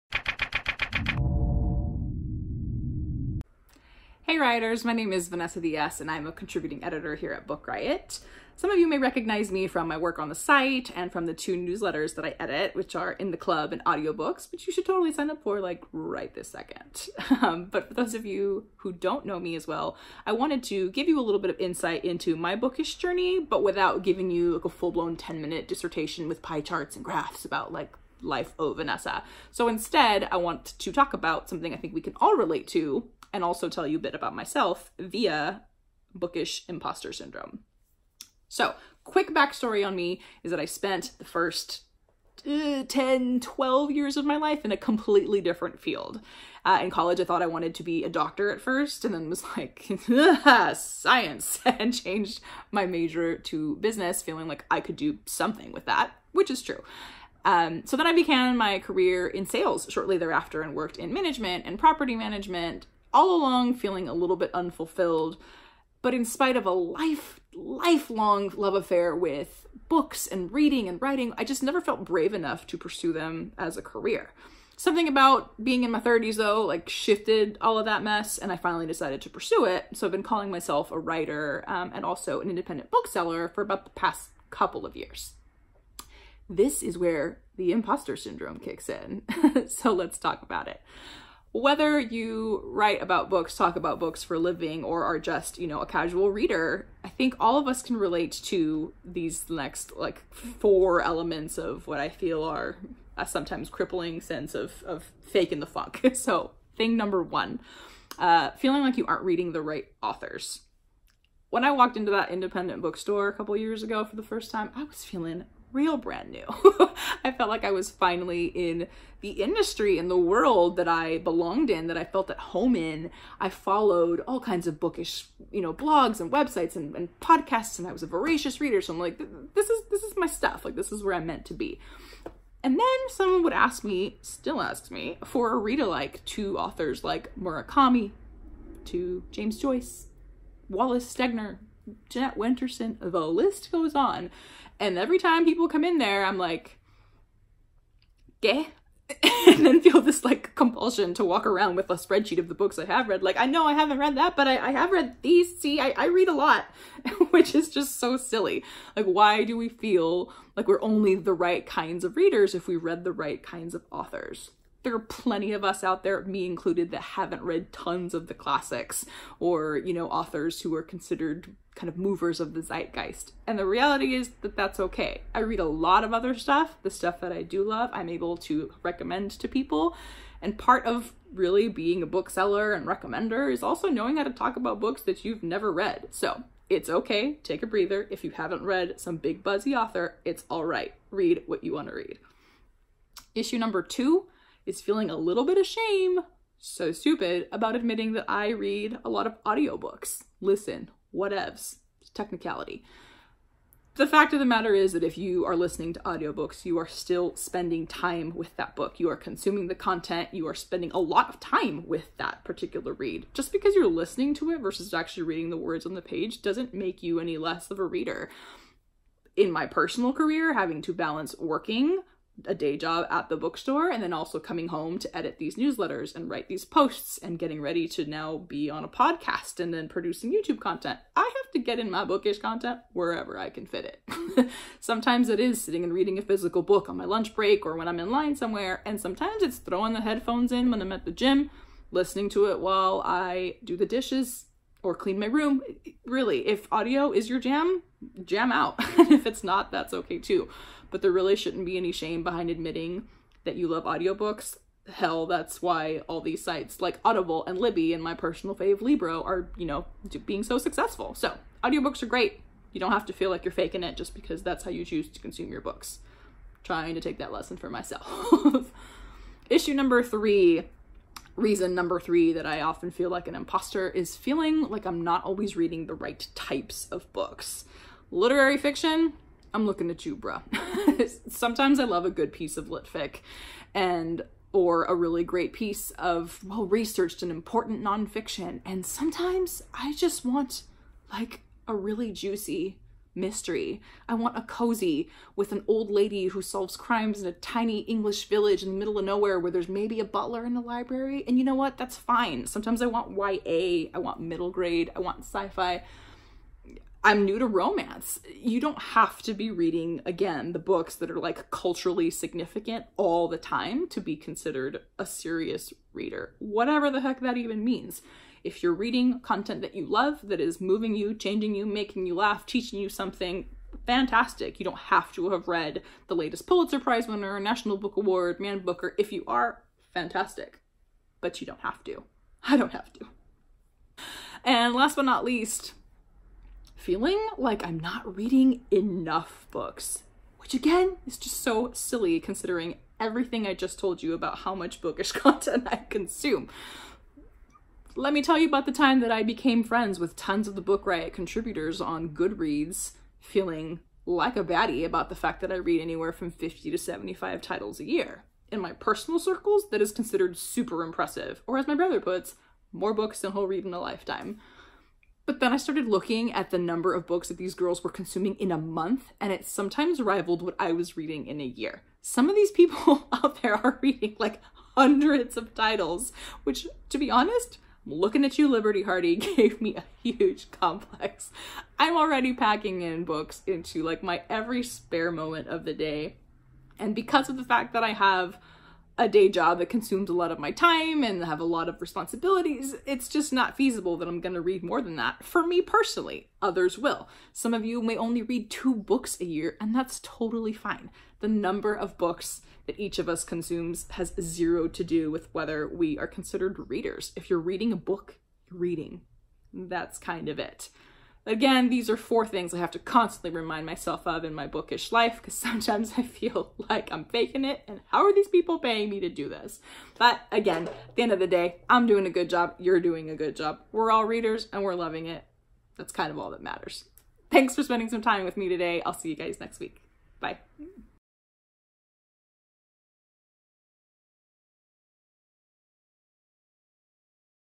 Confession: I'm a Bookish Imposter
Get to know Vanessa as she shares her experience battling imposter syndrome in her reading life.